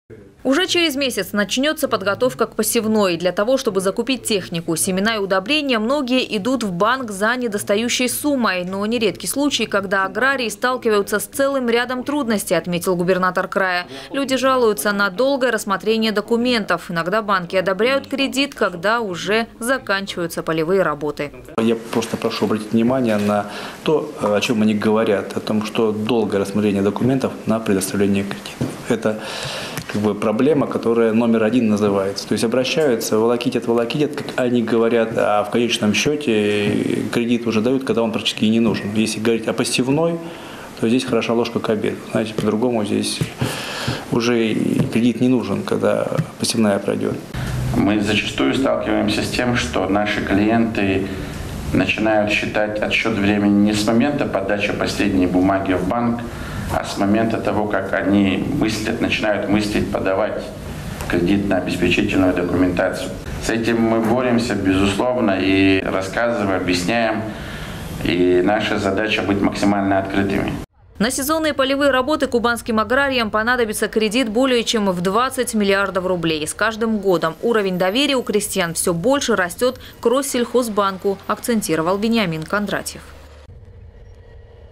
Кубанские аграрии ежегодно берут в кредит около 100 миллиардов рублей. Новости Сочи Эфкате
Потребность крестьян в банковских ресурсах Вениамин Кондратьев обсудил с руководителем краснодарского филиала "Россельхозбанка".
Сочи. Новости.
Телеканал Эфкате.